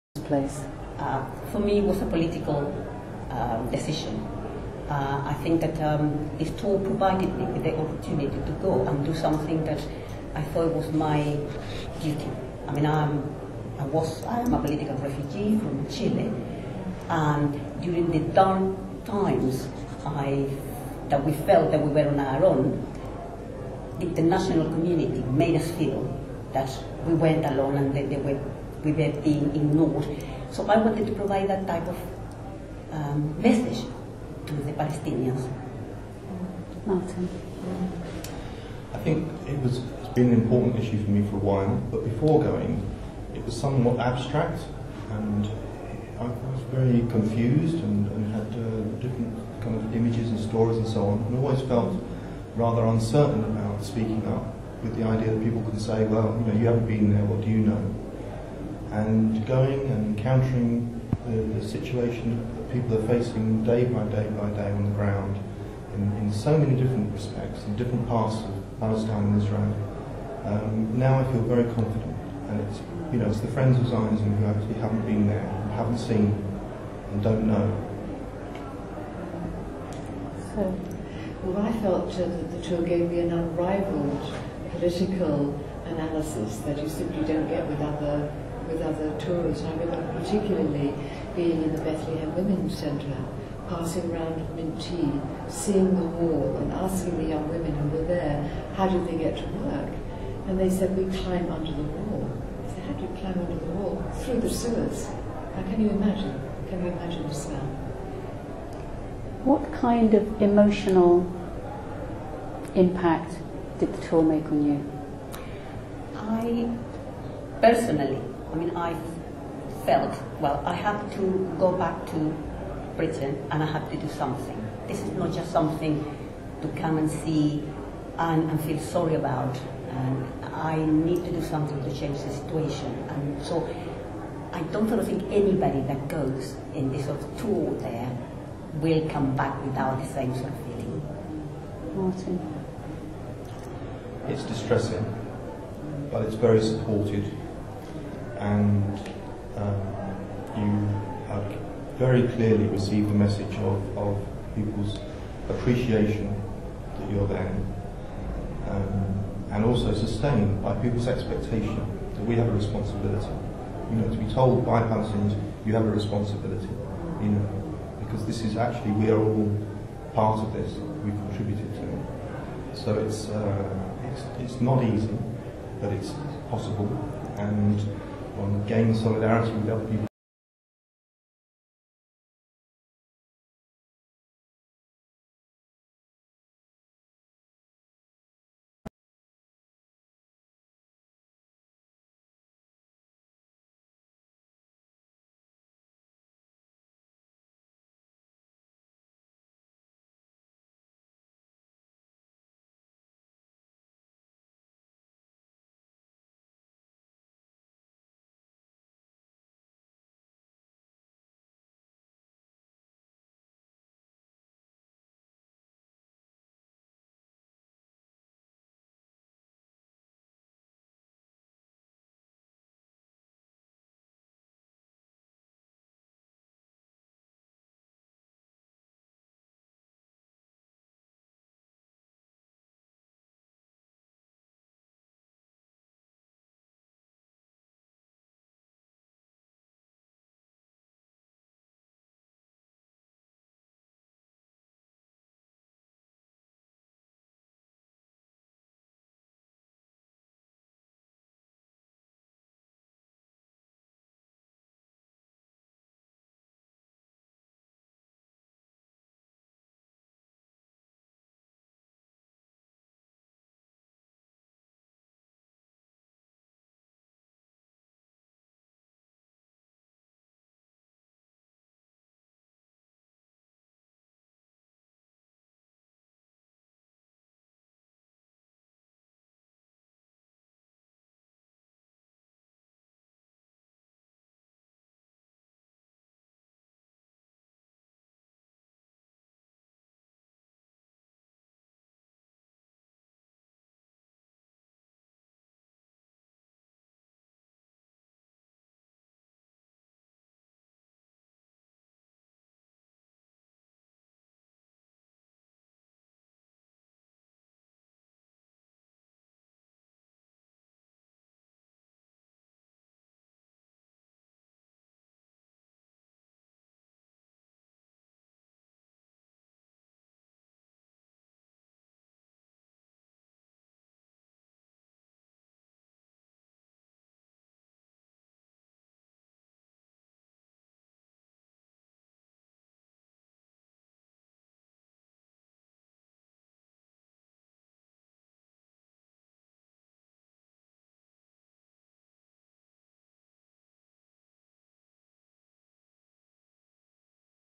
ICAHD Political Study Tours
Participants from ICAHD Tours talk about why they chose to take part and how this has affected them